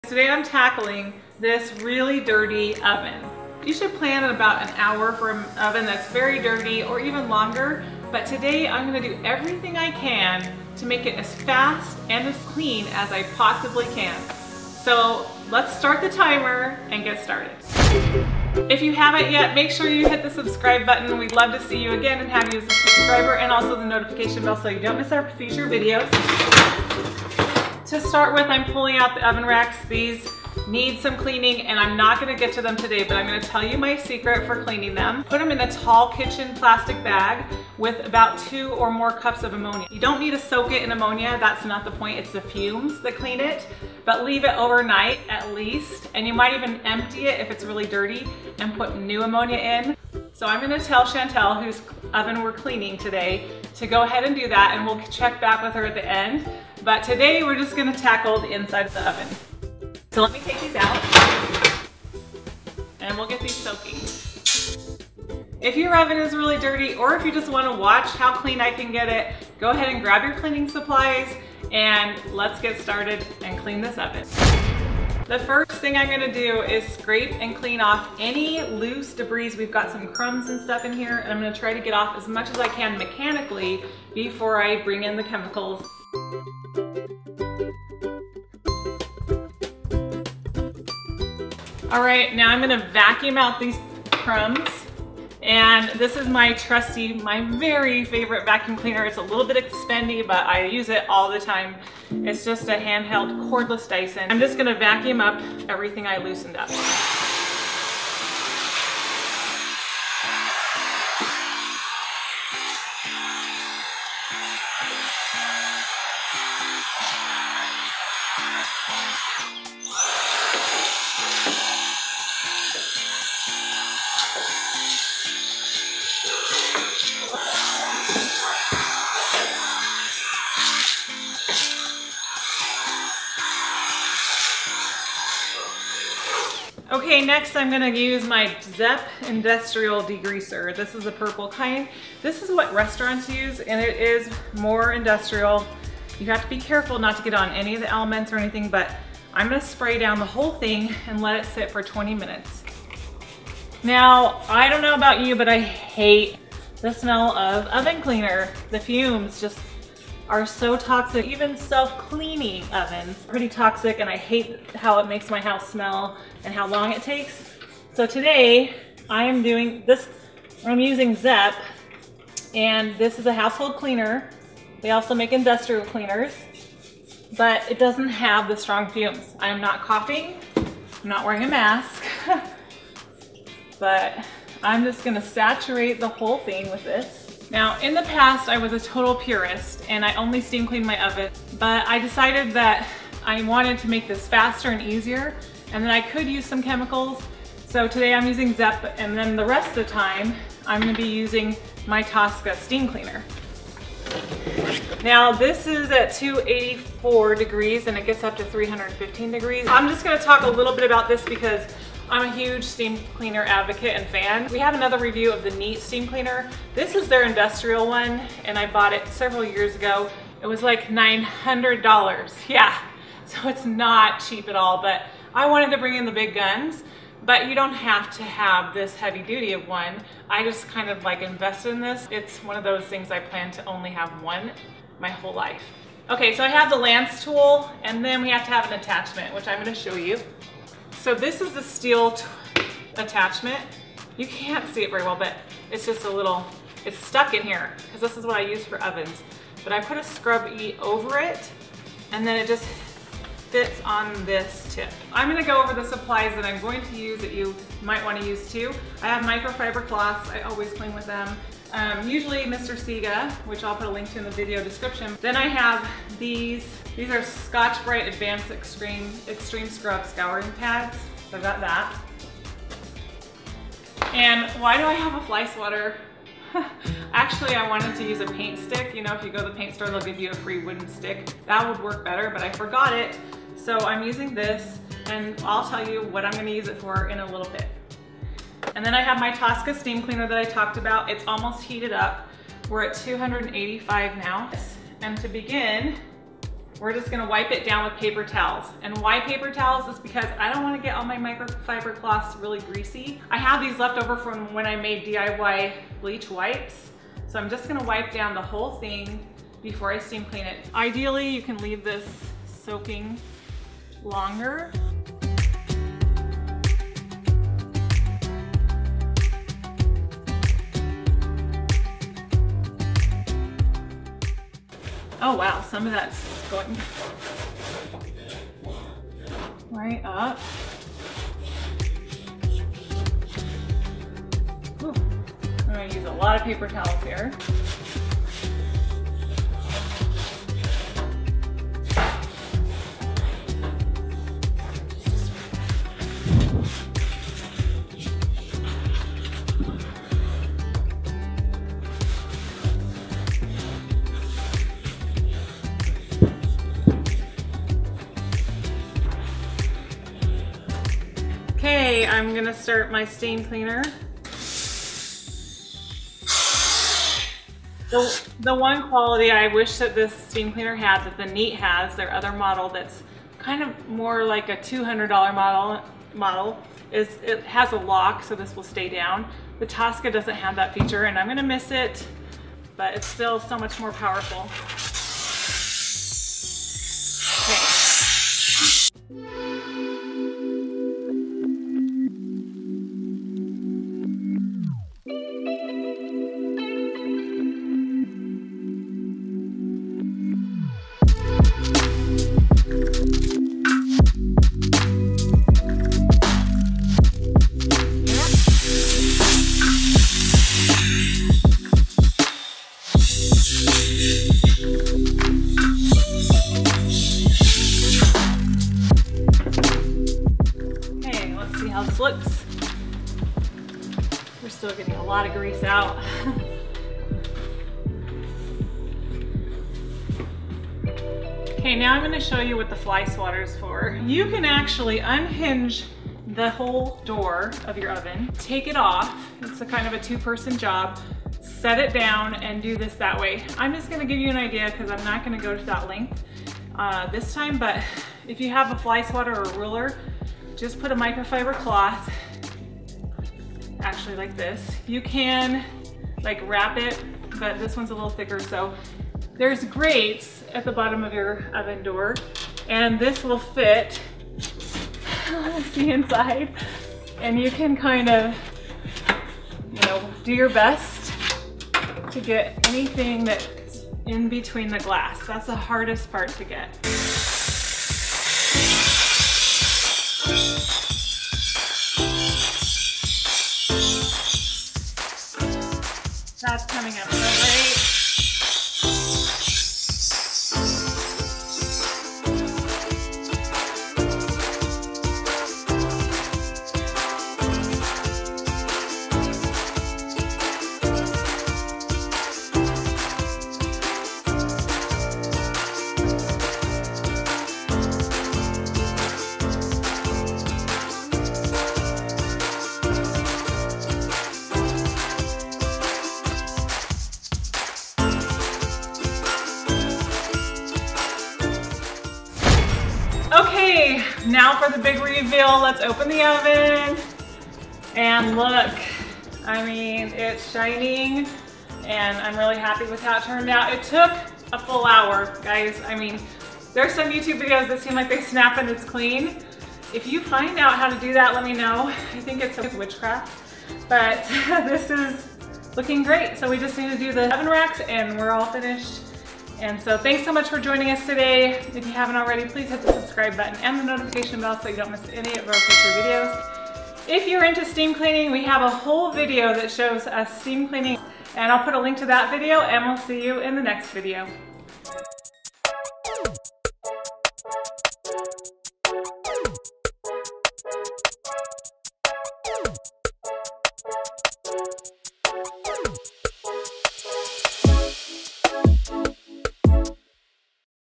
How to Clean an Oven with a Steam Cleaner
In each oven cleaning challenge we test different methods to find the fastest and easiest way to clean an oven. This is a tough job with lots of grease and baked on food that's been there for a long time. This tough job calls for the Dupray Tosca steam cleaner and Zep industrial strength degreaser!

Oven Cleaning PRODUCTS IN VIDEO:
Optional: a brush, this is a grout brush but it is stiffer than other brushes
➡️ Dupray Tosca Steam Cleaner

➡️ Zep industrial degreaser
➡️ Scotch-Brite Heavy Duty Scouring pads
➡️ Scotch-Brite Stainless Steel Scrubber

Step 1: Remove oven racks and treat them overnight with ammonia. Turn on steam cleaner to heat up.
Step 2: Scrape out any food particles or loose debris inside the oven with a stiff brush. Vacuum it up.
Step 3: Spray everything with Zep degreaser and close oven door. Let sit for 20 minutes.
Step 4: Use the steam cleaner with the steel brush attachment and stainless steel pad to clean.
Step 5: Wipe down with a microfiber cloth.

A dirty oven can easily take an hour or more to clean. Can you avoid caustic chemicals that burn your lungs and skin? Can I get it down to 30 minutes or less? Do natural cleaning solutions work? How about a steam cleaner? What's the best way to clean an oven? At the end of my experiments, I'll create a video with my favorite method.

I've cleaned a lot of ovens, but never with a mission like this one!

We did this test with a steam cleaner and degreaser because I hate fumes from oven cleaner or the smell/pain of self cleaning ovens.

In the past I was a purist who only used a steam cleaner but now I use all the tools I can. I didn't even use the attachments for the steam cleaner.

The Dupray Tosca cleans with water heated to 315 degrees (steam) and pressure without damaging surfaces. Dupray also has the Neat which costs a lot less but I pulled out the industrial Durpray for this big job.

If you hire out cleaning an oven it's going to cost you $30 or more. So think of it as you're saving $30 an hour when you do it yourself.

I used the industrial degreaser because restaurants use it and this oven is super greasy!